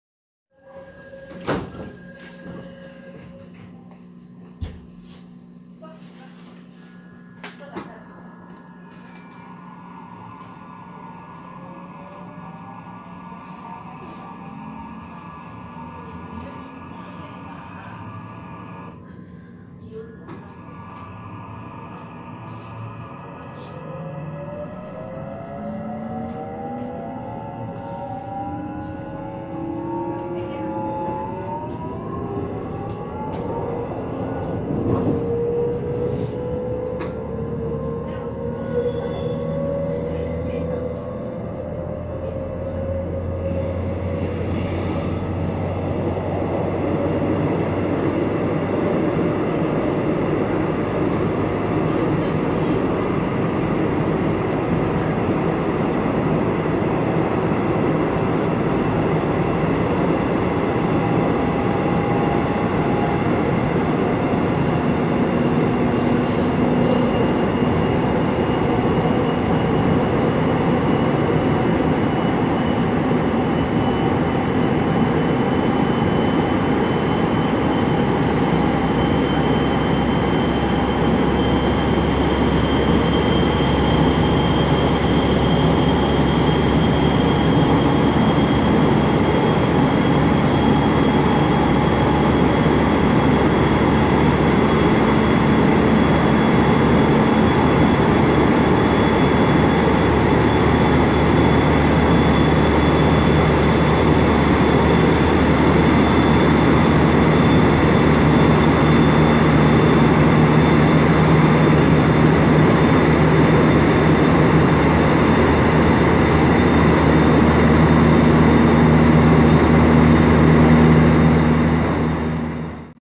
코레일 경강선(성남여주선) 가속음
여주발 판교행, 371×08 편성
(구간 : 신둔도예촌역 발차)